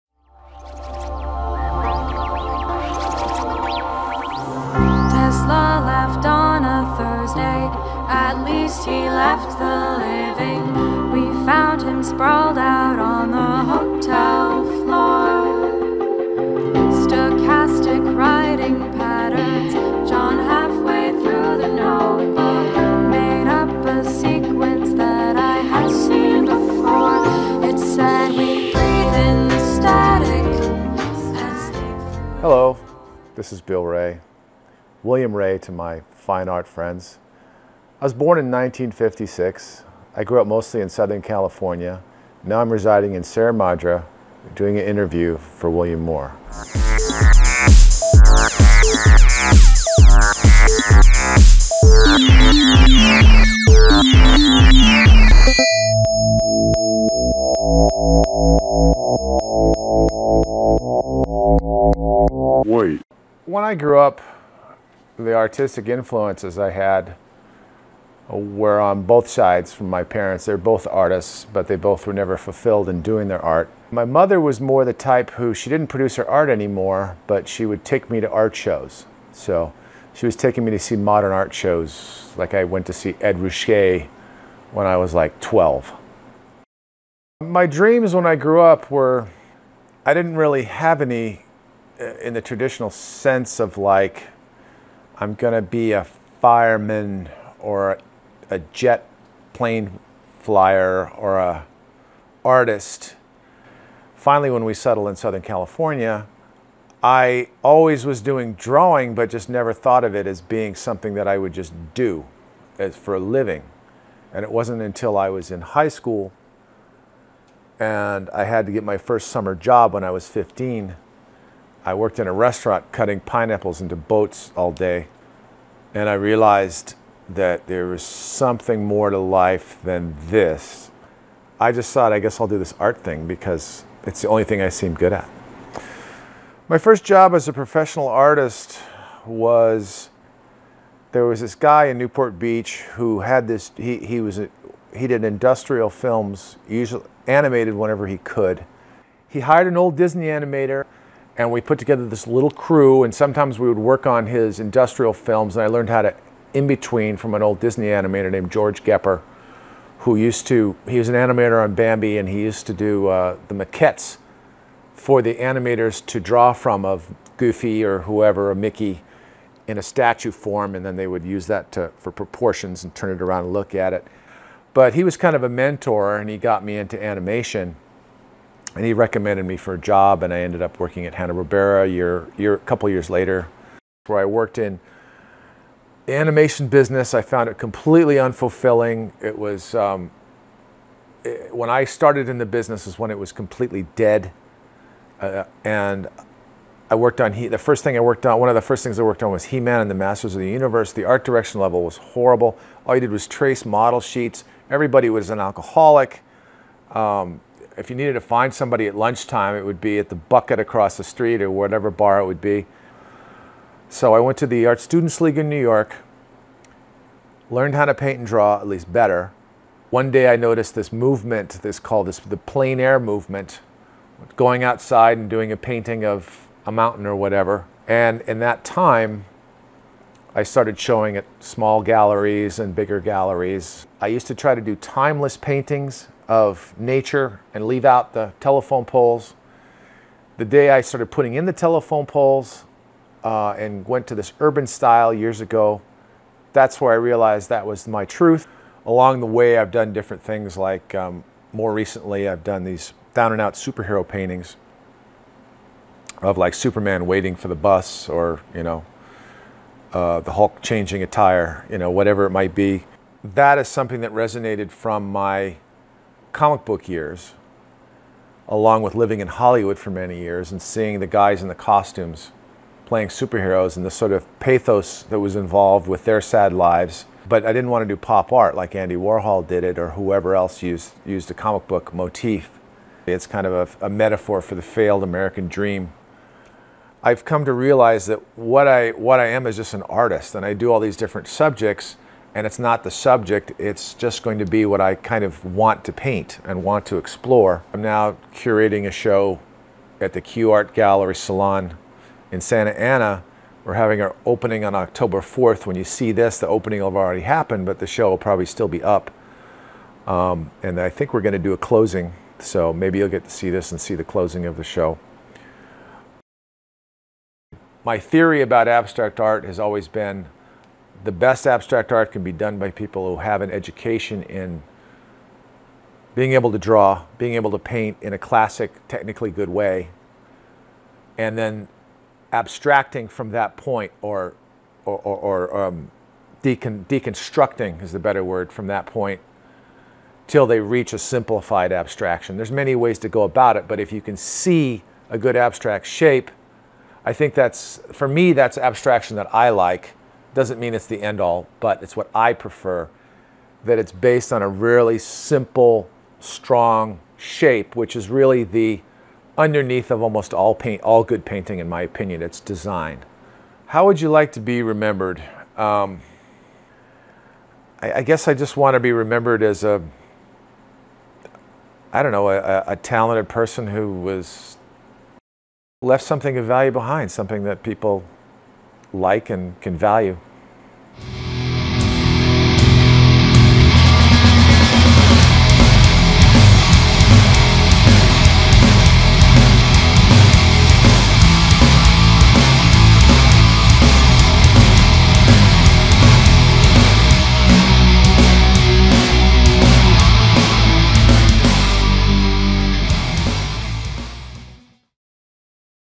William Wray Painter - Interview October, 2014
An interview with artist William Wray, living in Sierra Madre, CA.  William gives us a little background about his life and finishes with the announcement of a show he curated and is appearing in this Oct, 2014 at the Q Art Salon, Santa Ana, CA